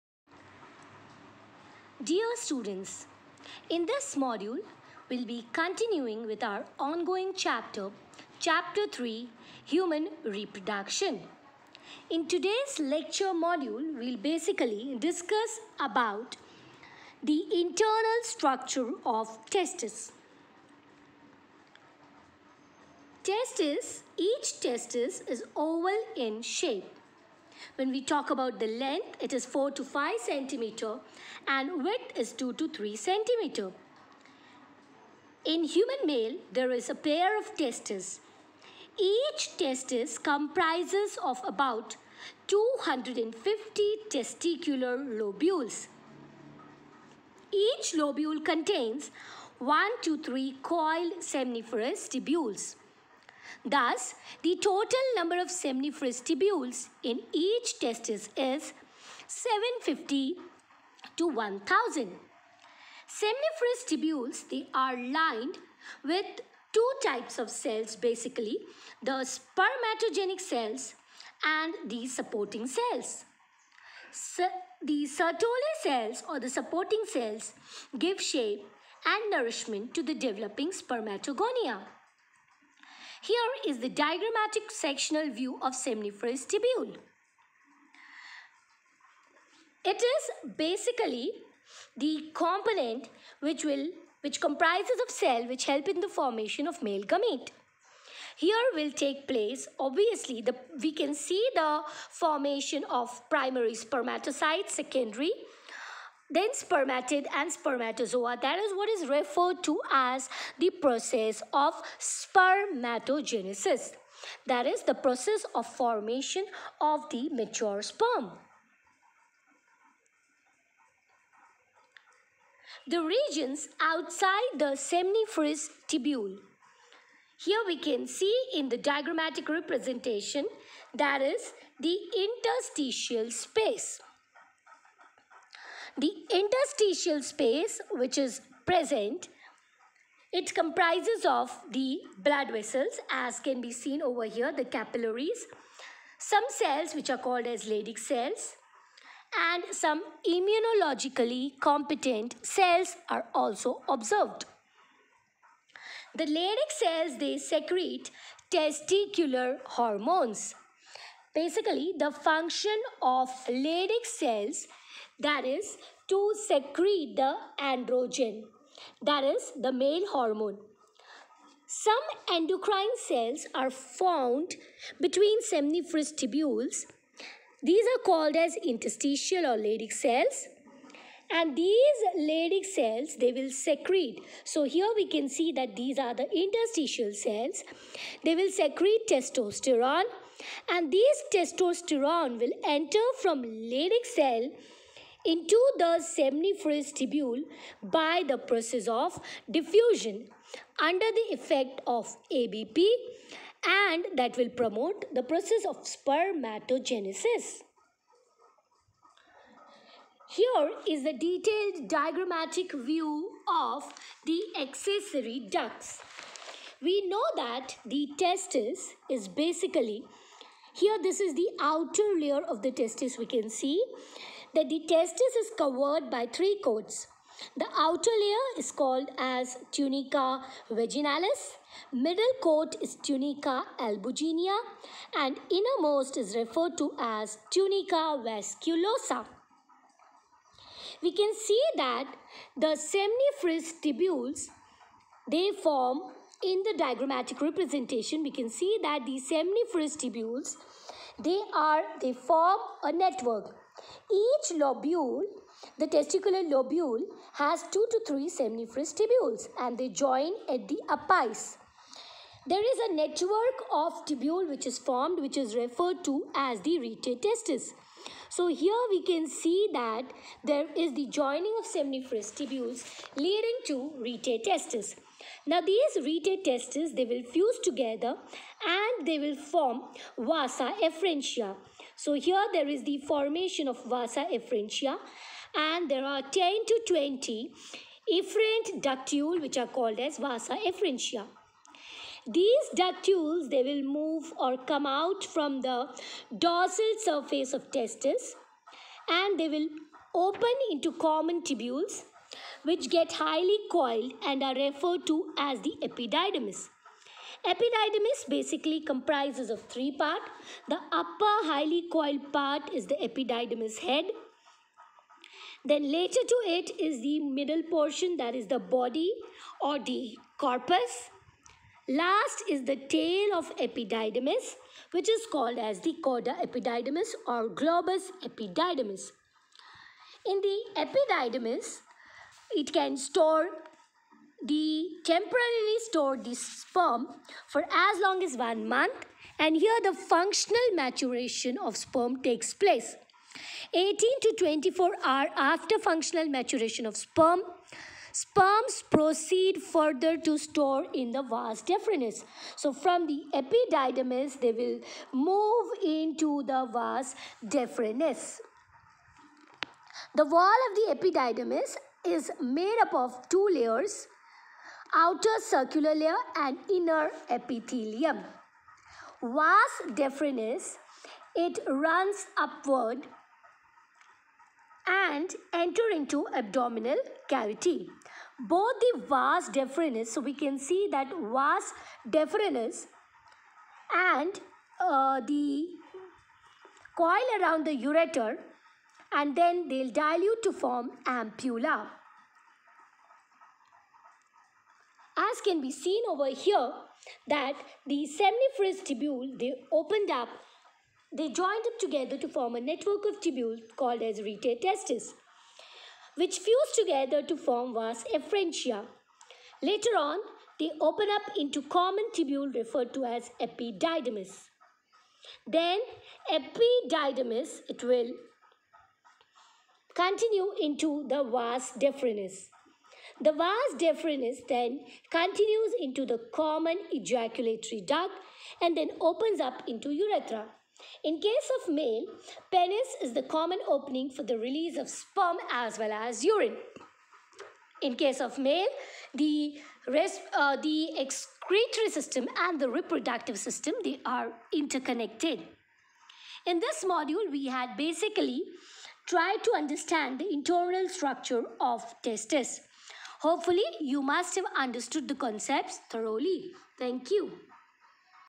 Internal structure of Testis by Deepti Joshi, KVM Public School, Haldwani
Biology, Class XII, Chapter 3 - Human Reproduction